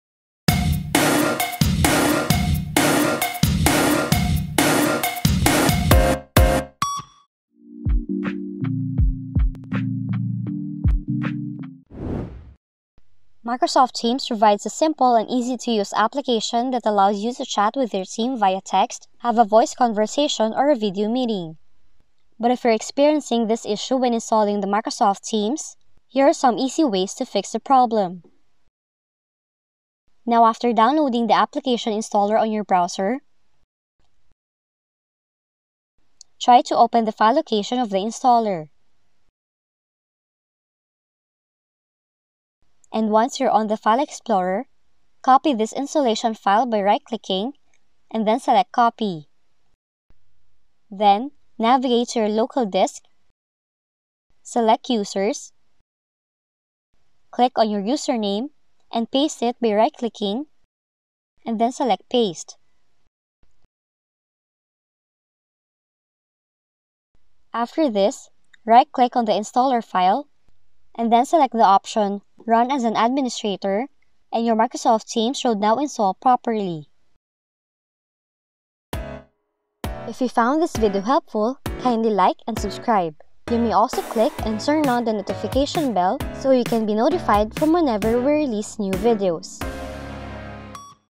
[SOLVED]: Can't Install MS Teams - IT Policy Restricting Access (error)
Are you getting this error when trying to install Microsoft Teams:

"Installation has failed, unable to write to C:\ProgramData\username - IT policies may be restricting access to this folder" ?

If so, follow the steps in this video to resolve this common issue!